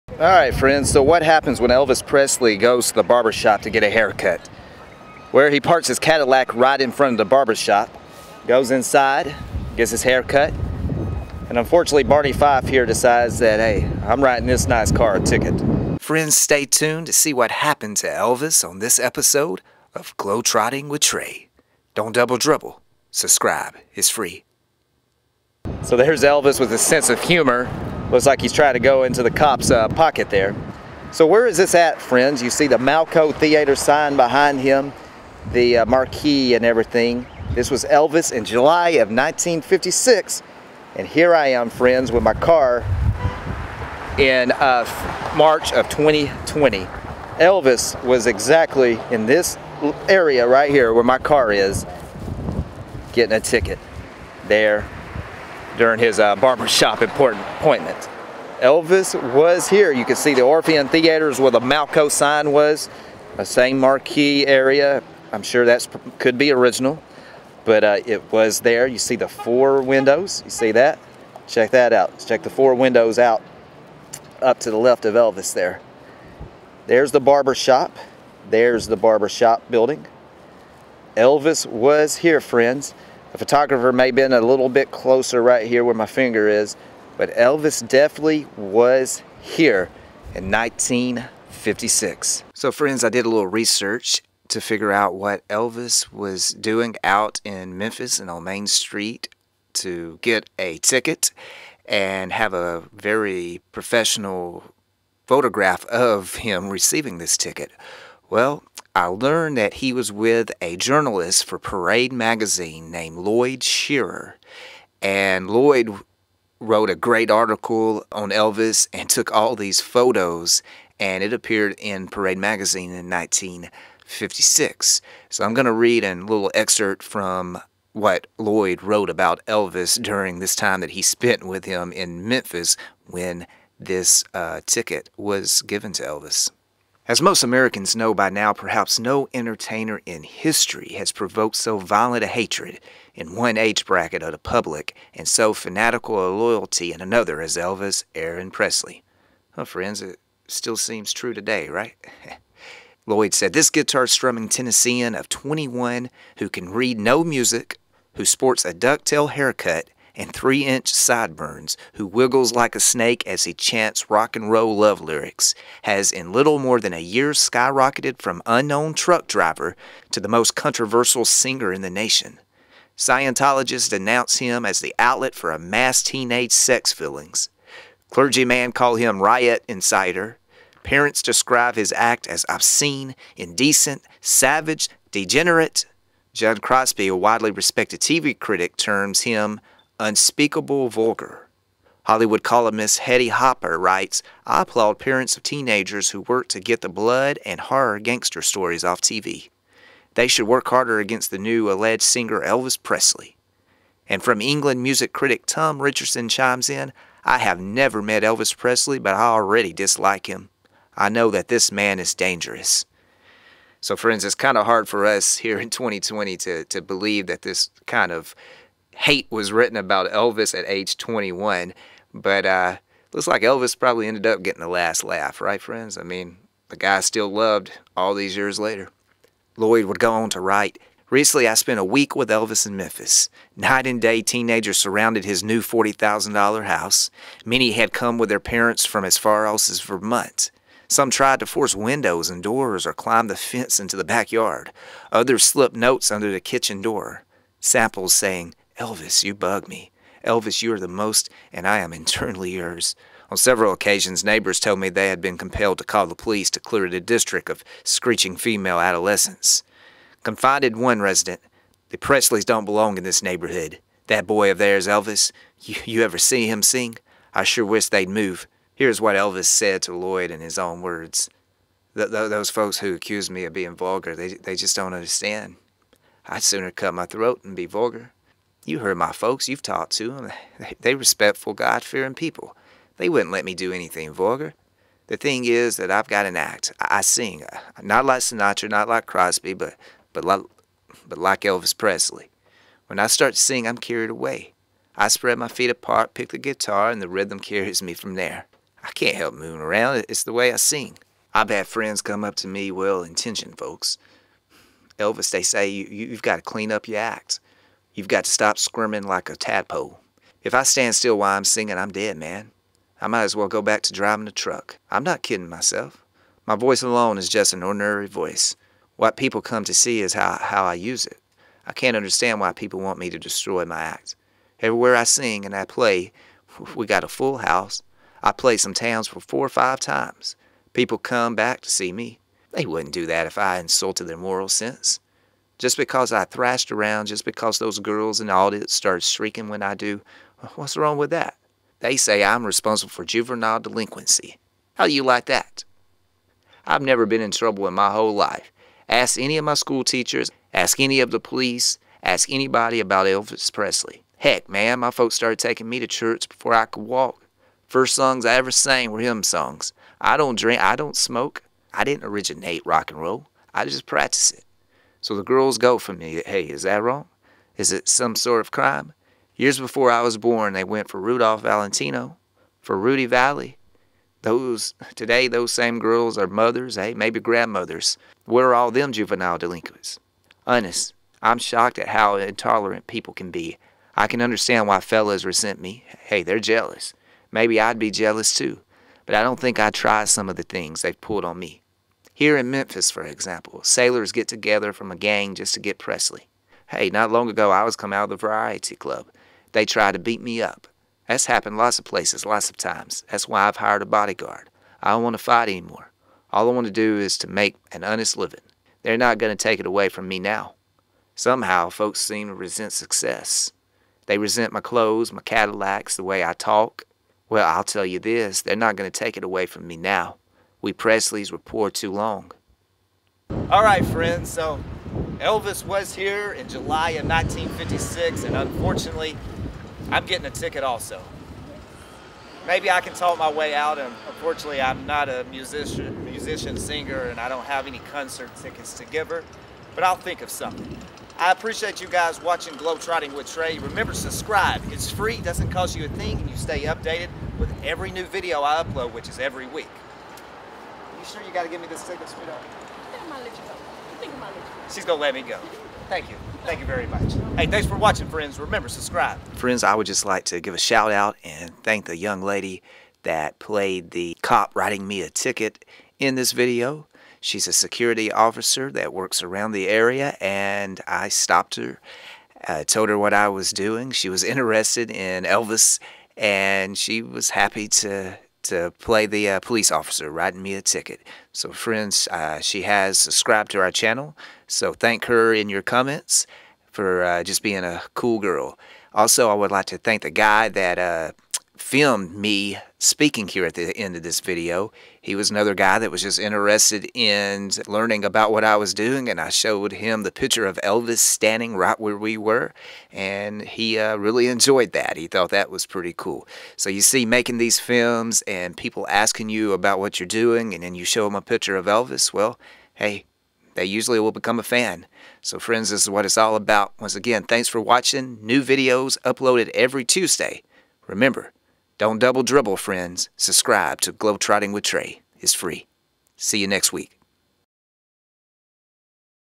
Elvis Gets a Ticket in July 1956
On episode #5 of "Globetrotting with Trey", Join me as I take you to a place in Memphis, Tn where Elvis Presley was written a parking ticket in July 1956! Plus I share an article that you probably have never heard!

(I appreciate your Support & all Donations will go to travel expenses to film shows)